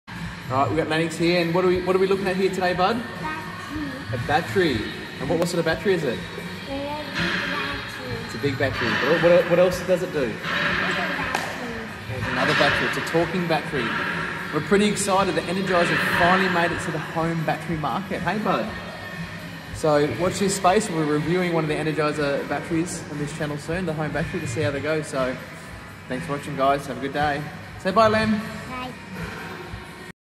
Energizer home battery storage
Energizer home battery storage had finally made it to the market. Looking forward to testing it at the bat lab!